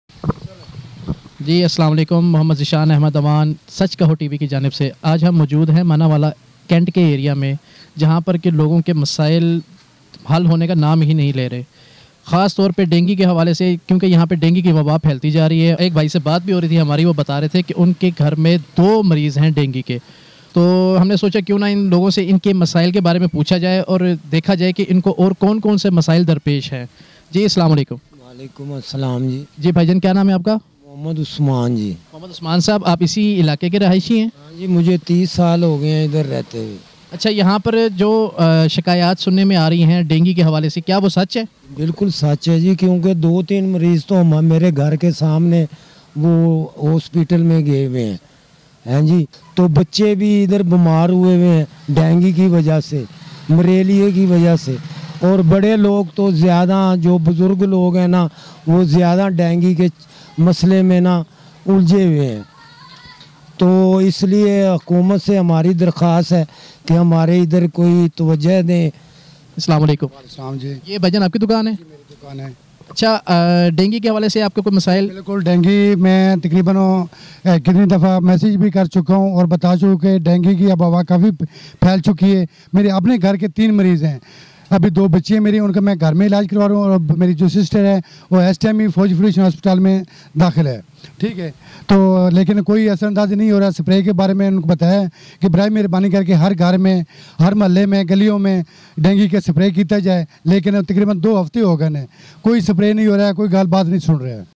Dengue Is Killing People Again. ڈینگی کا پھر سے خطرناک وار
Peoples are suffering from dengue problem.
Dengue health department is not paying attention to dengue problems.
People are furious against dengue health department negligence.

لوگ ڈینگی کے مرض میں مبتلا ہیں۔
محکمہ صحت ڈینگی کے مسائل پر توجہ نہیں دے رہا۔
محکمہ صحت ڈینگی کی غفلت کے خلاف عوام میں شدید غم و غصہ ہے۔